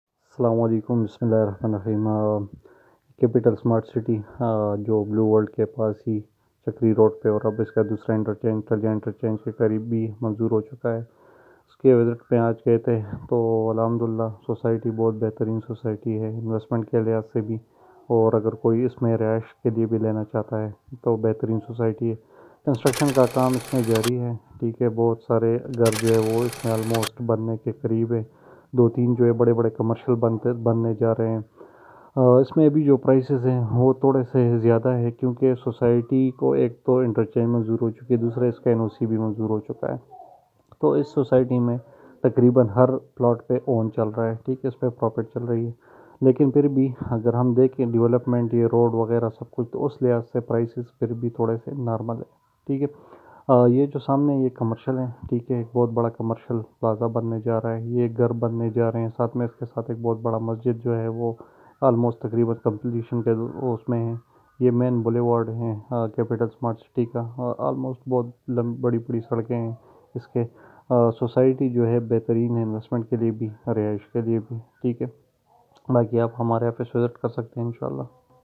Capital Smart City Islamabad//Best for Investment//Ideal location//Kp dream Property
Capital smart city is one of the finest modern design society of this era having residential and commercial plots of Different sizes on Cash as well as on Installment.
Capital smart city is an ideal location of chakri road as well as near to thalian interchange which will be operational soon In shaa allah.

Kp dream property is registered real estate company based in peshawae hayatabad as well as our second office is located on main university road.